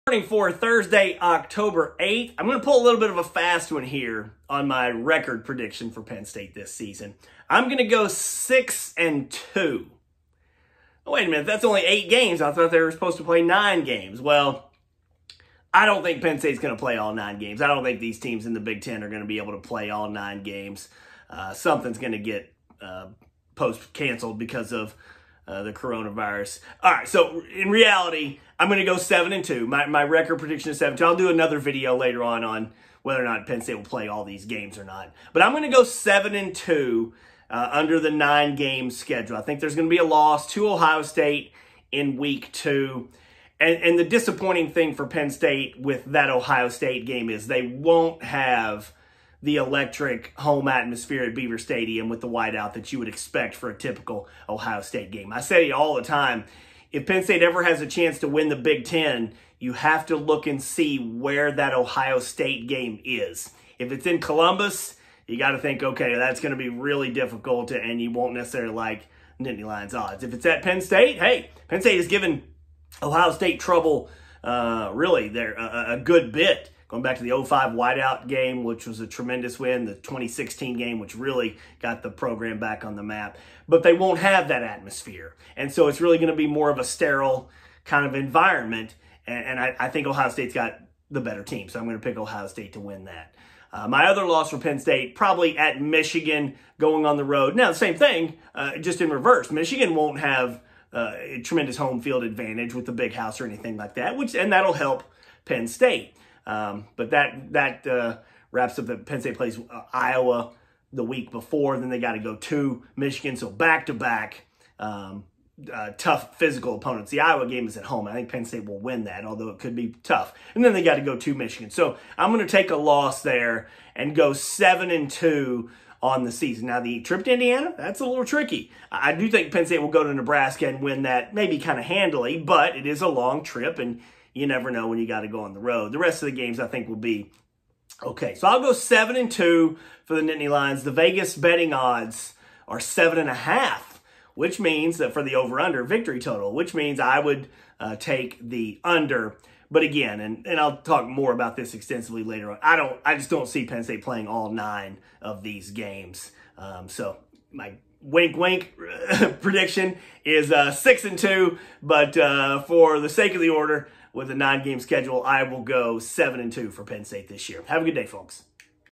2-Minute Morning (10-8): My Record Prediction for Penn State Is ... | NSN
Vegas has set PSU's over/under win total at 7.5, which means the Lions would have to finish 8-1 to cover. Will they?